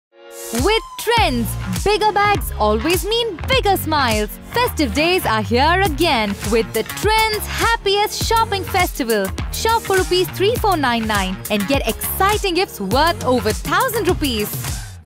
Trends Happiest Shopping Festival
A festive treat has come your way. Presenting the Trends Happiest Shopping Festival, offering exciting offers on your favourite styles. Shop for ₹3499 and get exciting gifts worth over ₹1000*, only at your nearest Trends store. It’s time to celebrate in style!

*Conditions apply. Please check with store staff for more details.